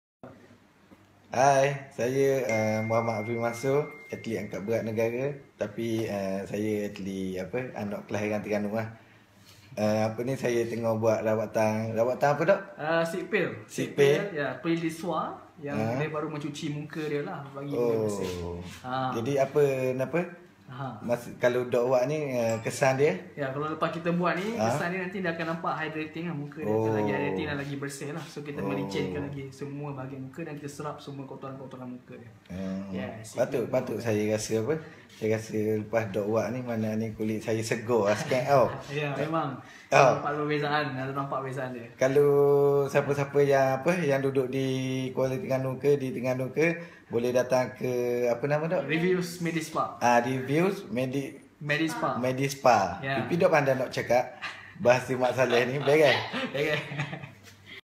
Hafifi Mansor - Atlet Angkat Berat Negara Buat Rawatan SILK PEEL di Reveuse Medispa
Atlet angkat berat negara hafifi mansor pn dah cube treatment PEEL DE SOIE atau silk peel